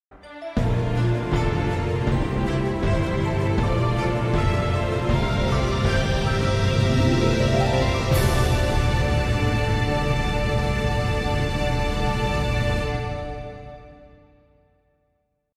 {REQUESTED} 2011gretel "NomNomNami 3.5" Logo (Gacha Club/The A/F Of September & November 2021 Ver.)
A/F = Autumn/Fall

Requested by: @2011gretel and @Yvan Chiari.

This Audio/Music is Used by Movie Star Pictures Company Limited Logo (2015).
(Pls, Resources to put Veg File or Vegas Pro 14 and Higher.)